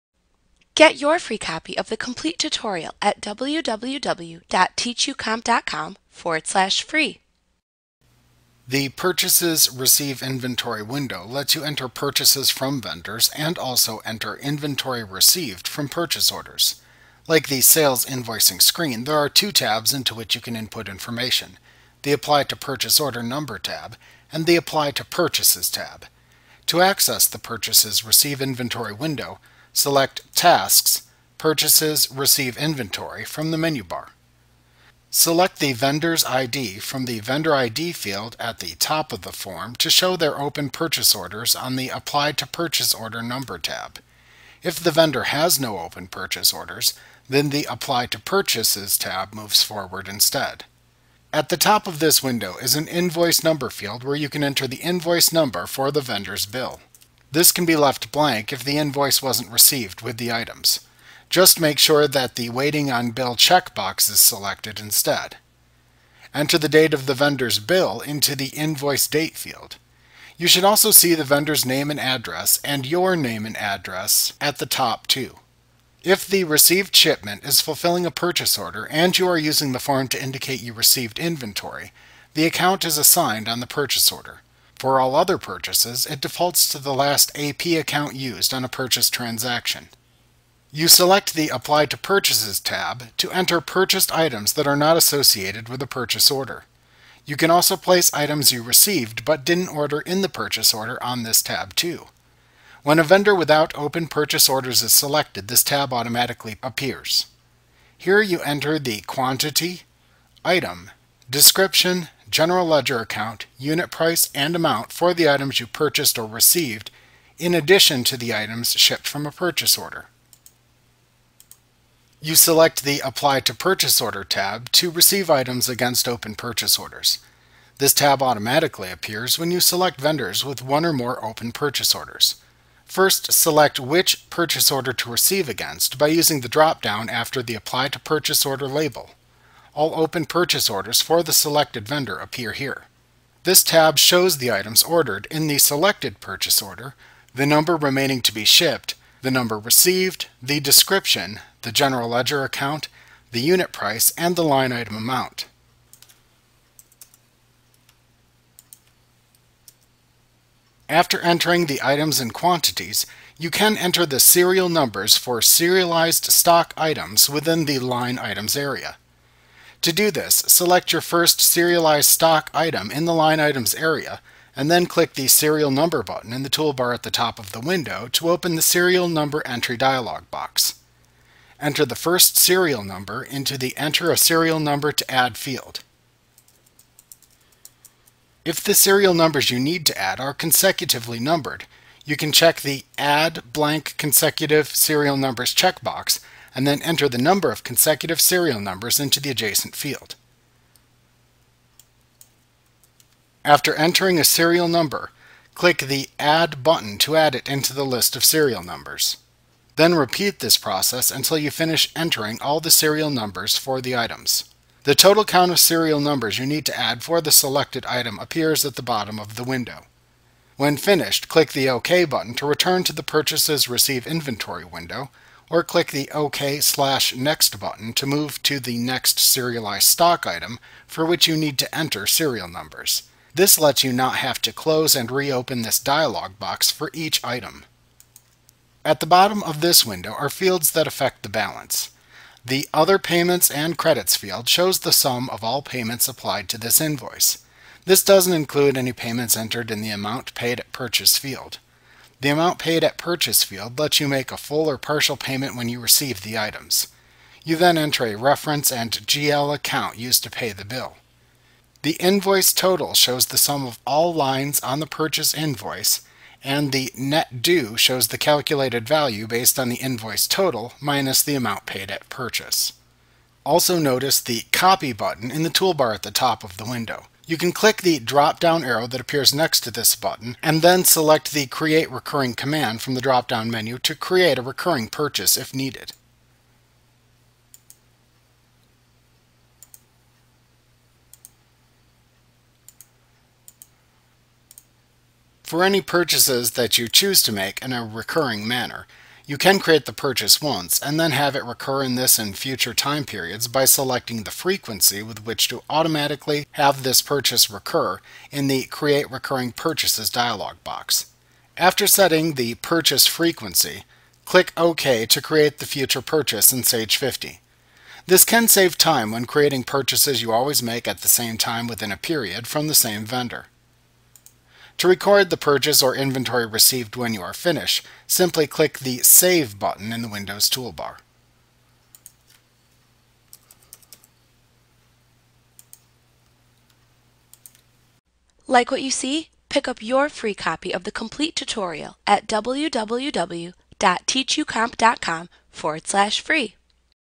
Sage 50 2018 Tutorial The Purchases/Receive Inventory Window Sage Training
Learn about The Purchases/Receive Inventory Window in Sage 50 A clip from Mastering Sage 50 Made Easy v. 2018. - the most comprehensive Sage 50 tutorial available. Visit us today!